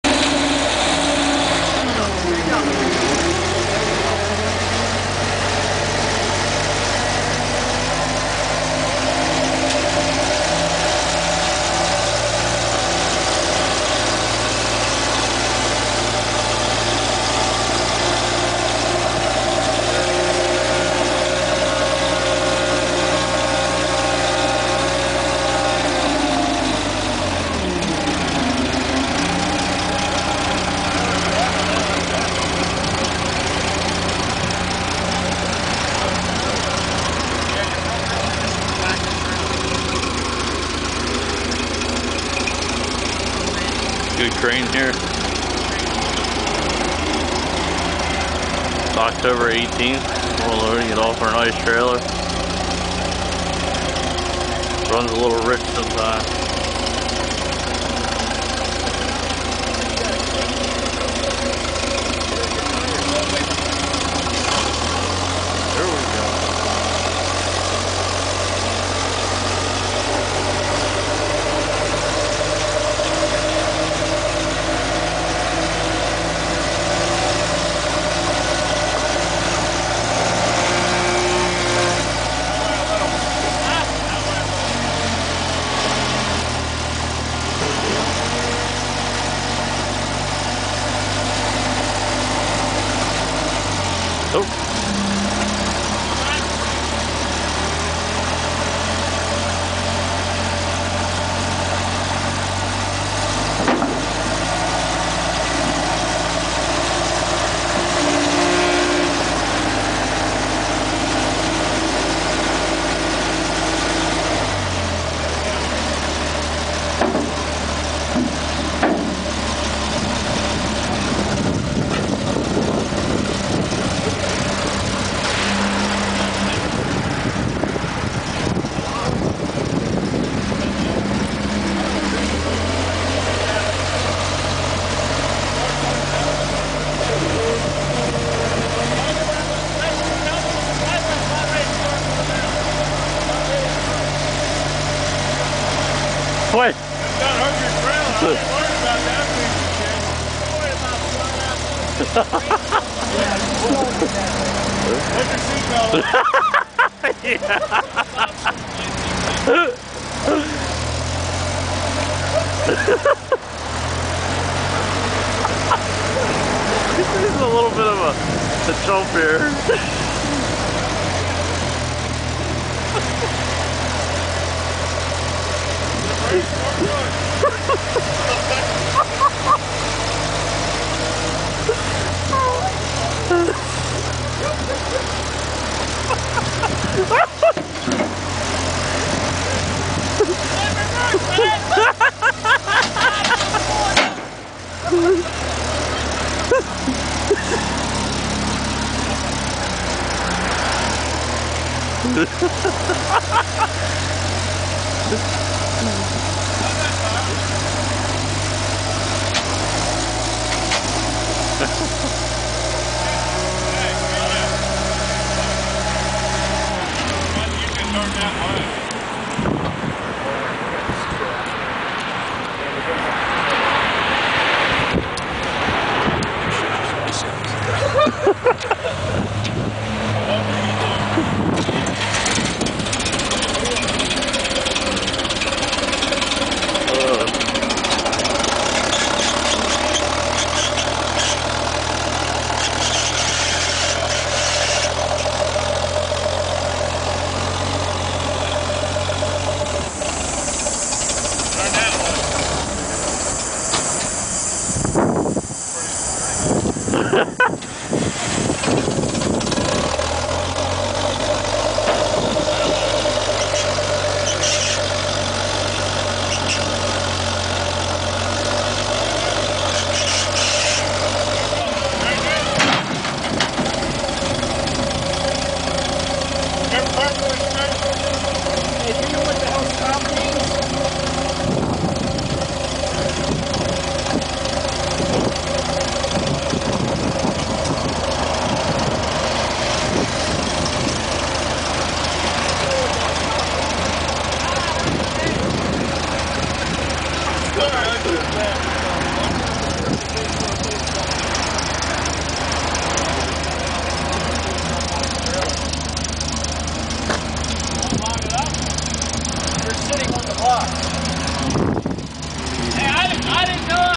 Drott Crane 4WD Unloading from trailer
We bought a Crane that was Shipped in for our towing and repair company